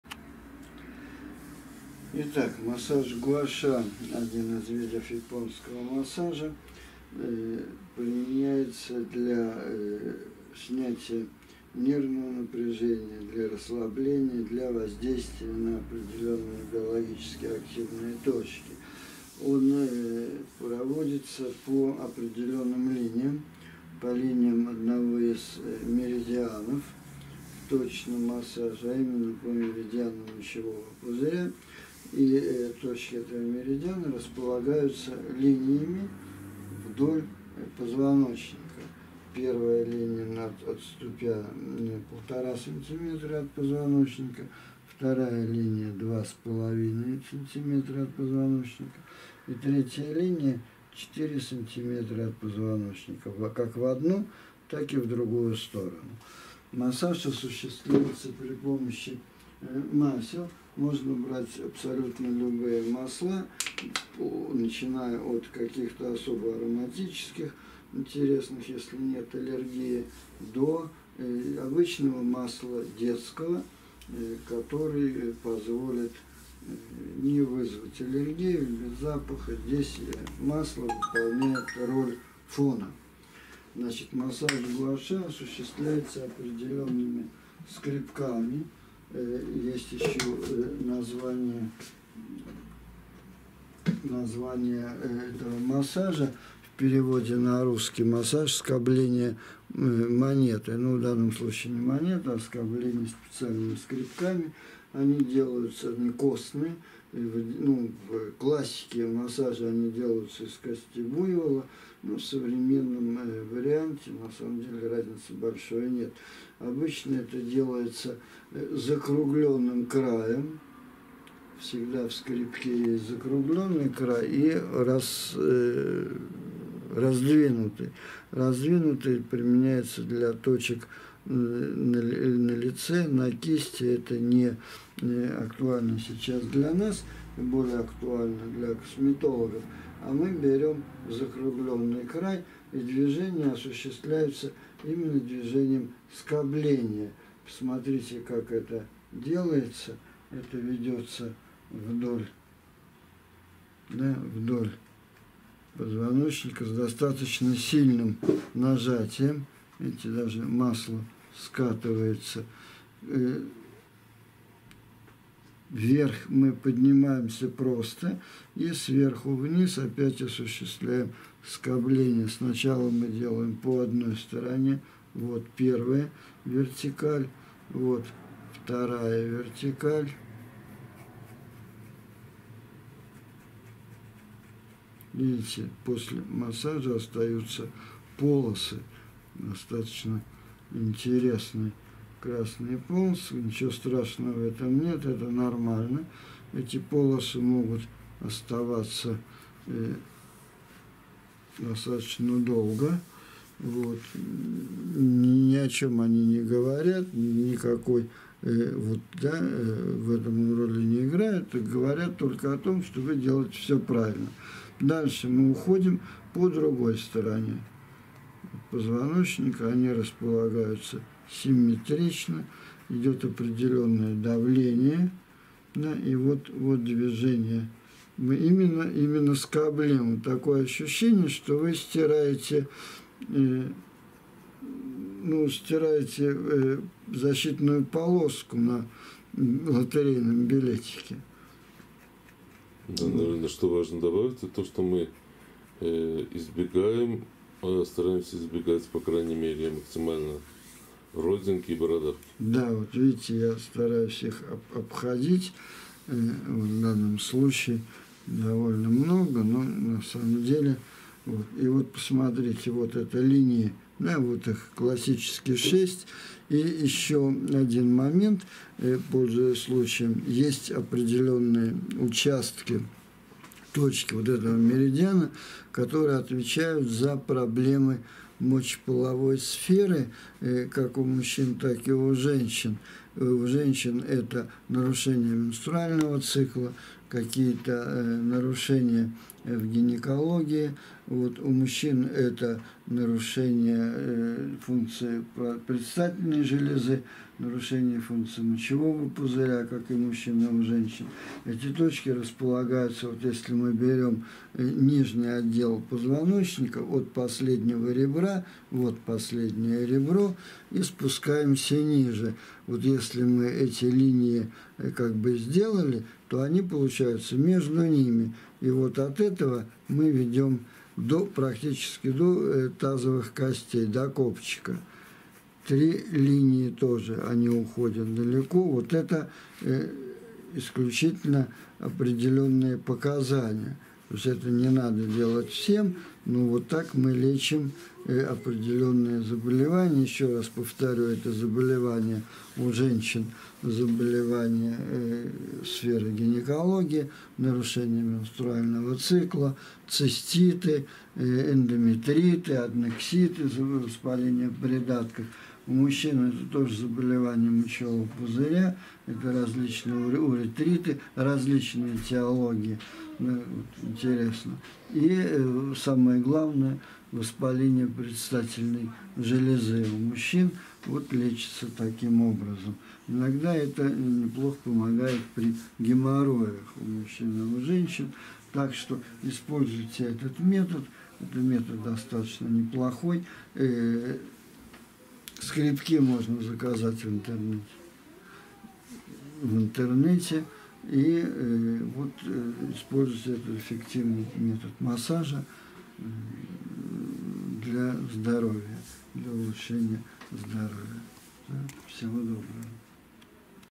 Массаж ГУАША для взрослых. Практикум.
Центр "БЕЭР ДАВИД" (Иерусалим).
Специалист Центра, доктор А.Тверской, показывает на практике массаж "гуаша" - один из видов традиционного китайского массажа.
Проводится точно по точкам меридиана мочевого пузыря, точки по линиям вдоль позвоночника. Используются специальные скребки, мази, кремы и масла.
Один из немногих видов массажа, целенаправленно "запускающих" регенерационные процессы в организме.